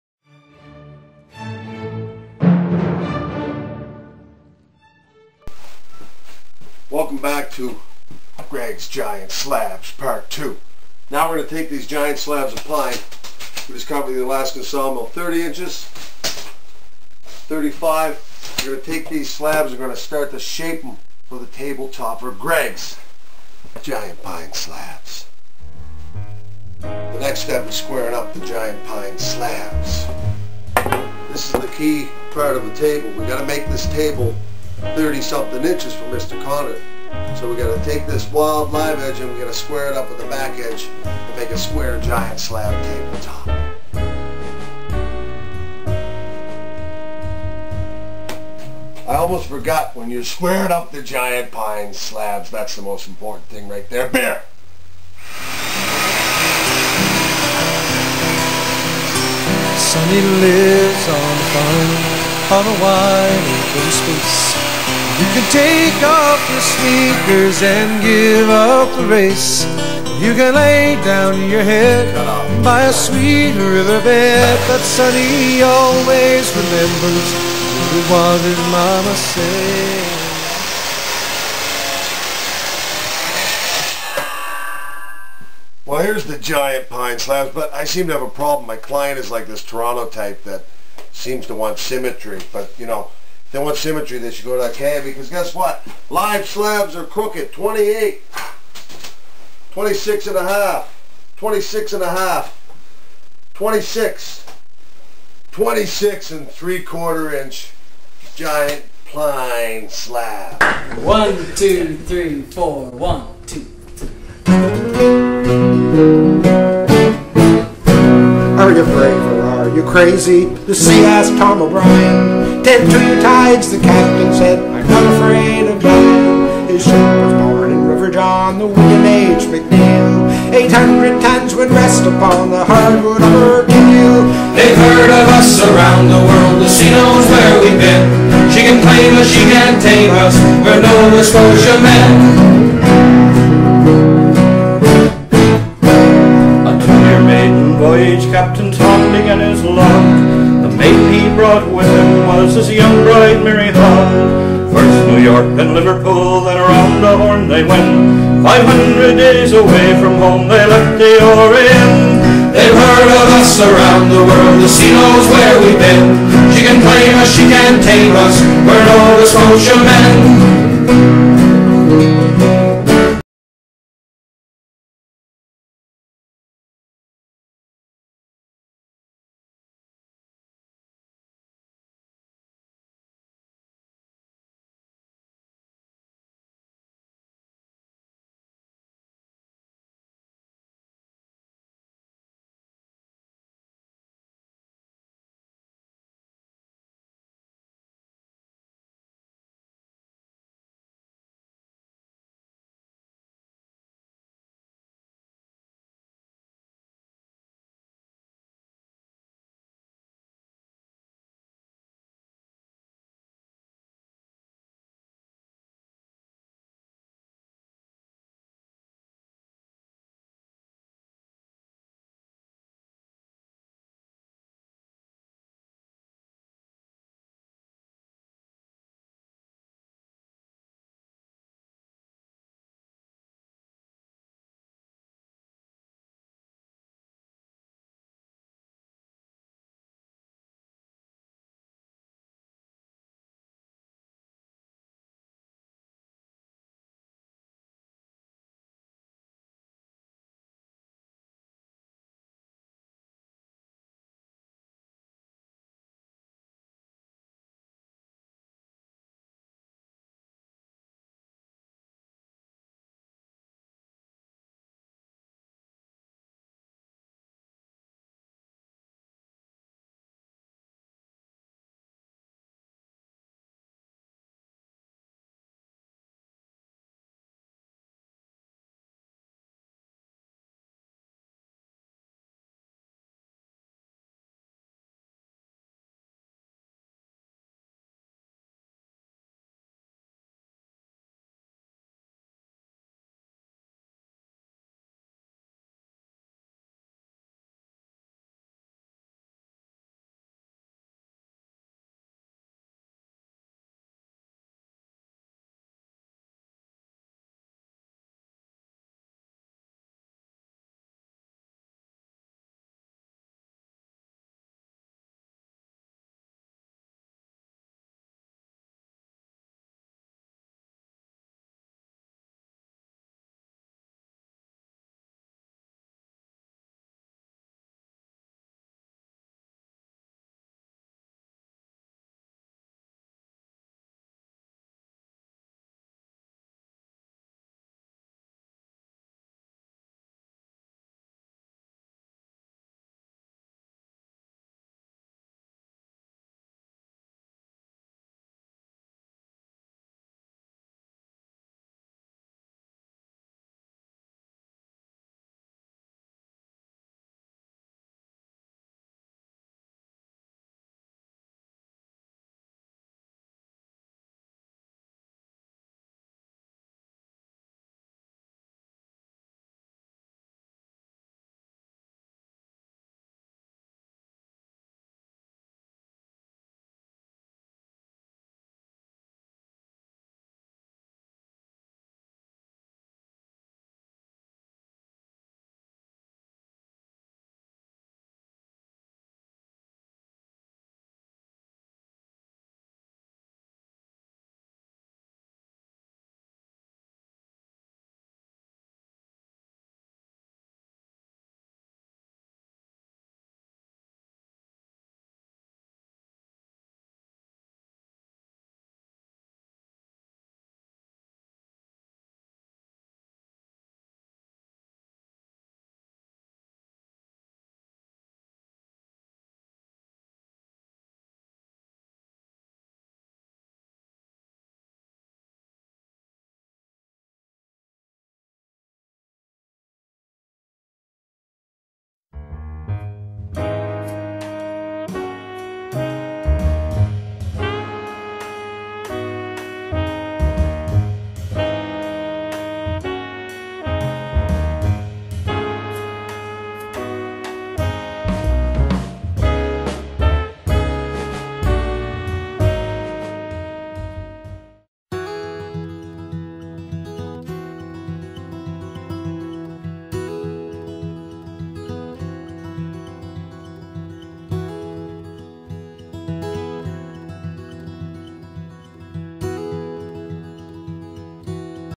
PART 2: Greg's Giant slabs. Actor/Artist Mark Owen squares the slabs.
Making of a large pine slab Rustic dining table with butterfly hinges.   This is part one of and eight part series on building a table top from rough slabs of pine. Nova Scotia  Actor and Artist  Mark Owen takes you from the first cut with the Alaskan sawmill to the final coat of stain on these Giant Pine slabs.  The end result is a gorgeous rustic LIVE EDGE dining room table top that will be shipped to Ontario.